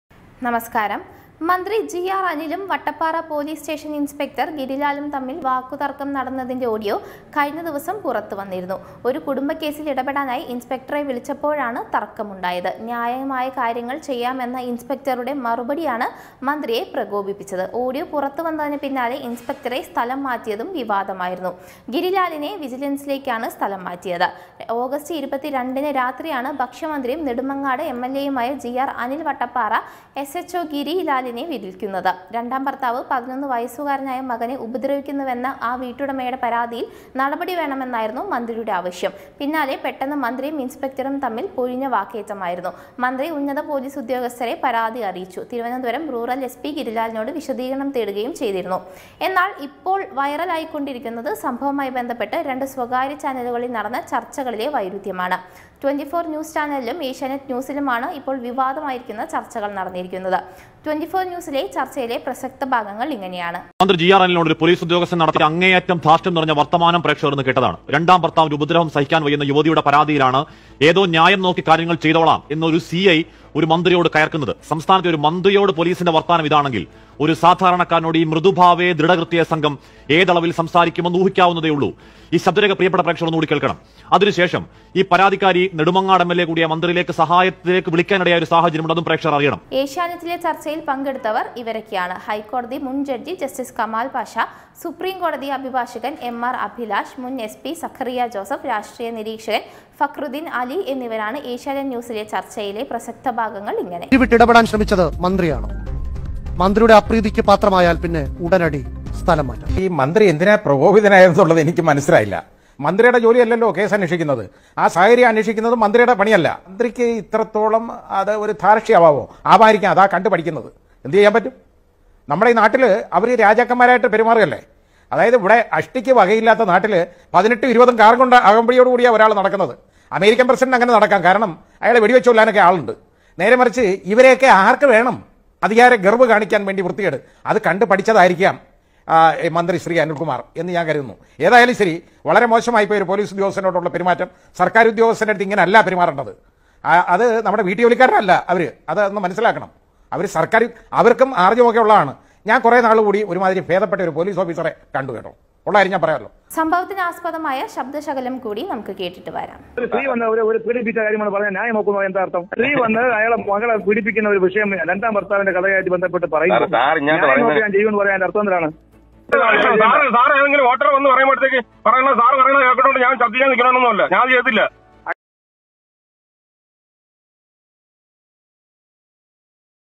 ഒരേ വിഷയത്തിൽ രണ്ട് വാർത്തകൾ
മന്ത്രിയും പോലീസ് ഉദ്യോഗസ്ഥനും തമ്മിലുണ്ടായ തർക്കവിഷയത്തിൽ മലയാളത്തിലെ രണ്ട് പ്രമുഖചാനലുകൾ ചെയ്തിരിക്കുന്ന വ്യത്യസ്തങ്ങളായ വാർത്ത കാണാം